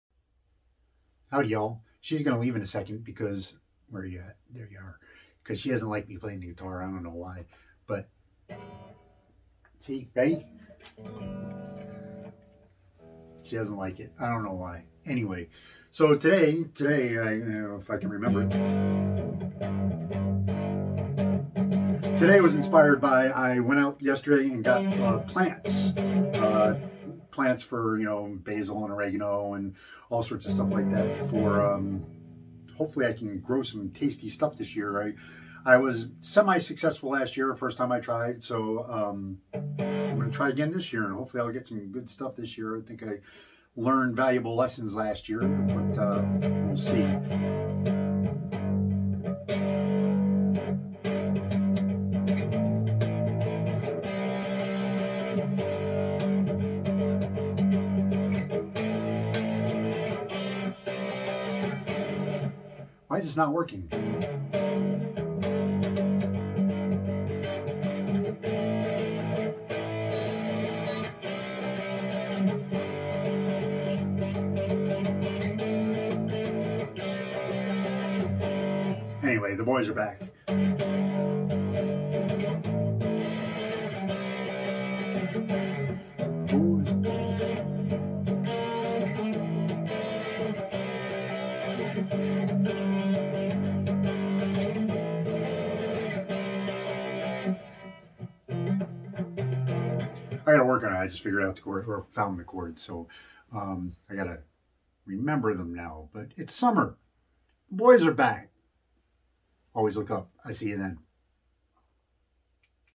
The Boys Are Back?
Well, I bought some plants... gonna try farmer Mike again...